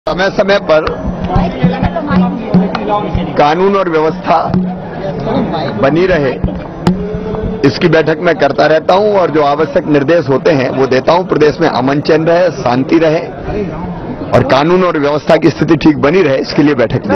शिवराज : कानून-व्यवस्था बनाए रखने के लिए कदम उठाए
* मध्यप्रदेश के मुख्यमंत्री शिवराज सिंह ने कहा, प्रदेश में कानून-व्यवस्था

और शांति बनाए रखने के  लिए कदम उठाए।
* अपराधों पर नजर बनाए रखने के‍ लिए नियमित रूप से बैठक।
* बैठक में अपराधों पर नियंत्रण के लिए ली जाने वाली एक्शन पर चर्चा।